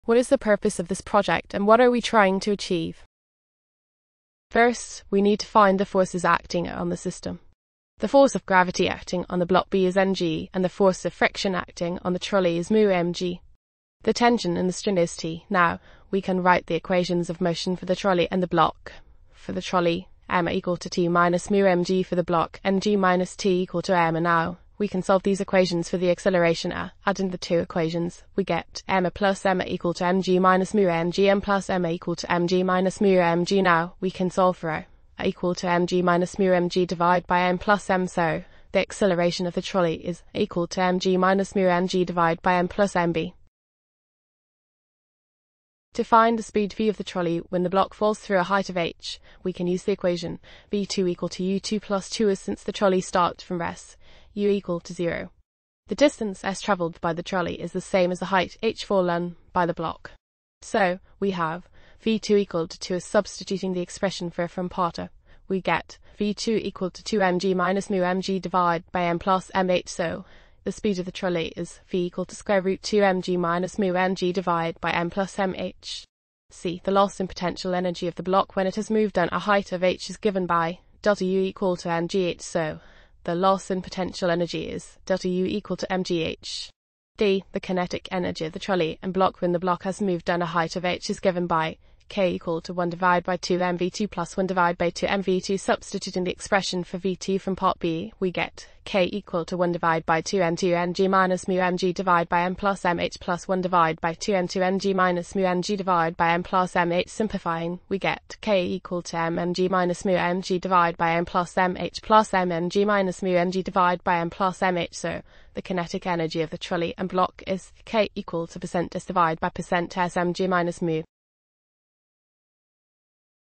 A trolley of mass m is connected to a block B of mass M by a string that passes over a smooth pu
A trolley of mass \( m \) is connected to a block B of mass \( M \) by a string that passes over a smooth pulley. The mass \( m \) is released, and it accelerates from rest. The coefficient of friction between the trolley and the horizontal surface is \( \mu \).
a. Derive an expression for the acceleration of the trolley in terms of \( m, M, \mu \) and \( g \)
b. The block falls through a height of \( h \). Deduce an expression for the speed \( v \) of the trolley
c. Write an expression for the loss potential energy of the block when the block has moved down a height of \( h \)
d. Write an expression for the kinetic energy of the trolley and block when the block has moved down a height of \( h \)